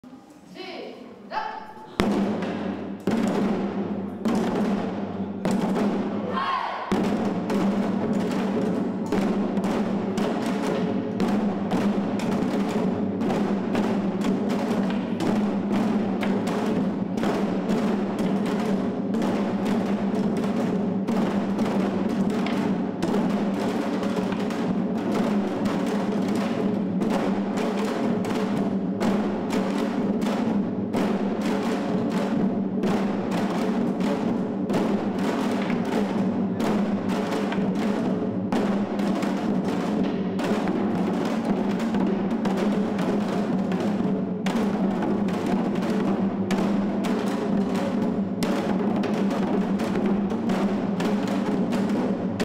Delhiites are fast learners: Samulnori Workshop
Ms Park Eun Ha shows Samulnori techniques at the workshop in the Korean Cultural Centre in Lajpat Nagar, Delhi.

Samul nori is a genre of traditional percussion music originating in Korea. The word samul means "four objects" and nori means "play"; samul nori is performed with four traditional Korean musical instruments:

    Kkwaenggwari (a small gong)
    Jing (a larger gong)
    Janggu (an hourglass-shaped drum)
    Buk (a barrel drum similar to the bass drum)

The traditional Korean instruments are called pungmul.

According to the Ministry of Culture, Sports and Tourism, the Korean Cultural Center located on Ring Road, New Delhi, is four stories tall, covering 1,826 square meters of floor space. The center has an AV room where visitors can watch video materials and an exhibition hall, and plans to offer Korean language, taekwondo, and traditional music classes.

This is the first Korean Cultural Center to be opened in South Asia and the 24th in the world since the first centers opened in Tokyo and New York in 1979.

Source: Wikipedia, Korean Culture. org